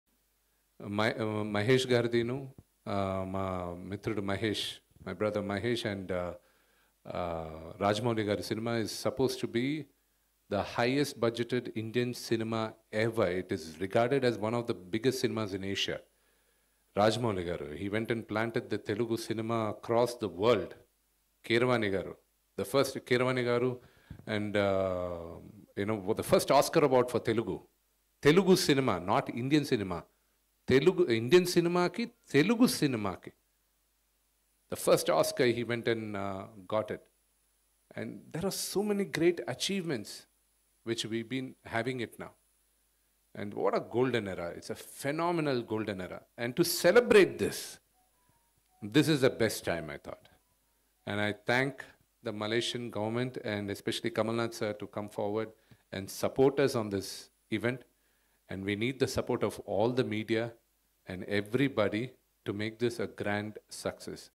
Manchu Vishnu Comments On Mahesh Babu & Rajamouli Movie | SSMB29 | Manastars
Watch ManchuVishnu Comments On MaheshBabu & Rajamouli Movie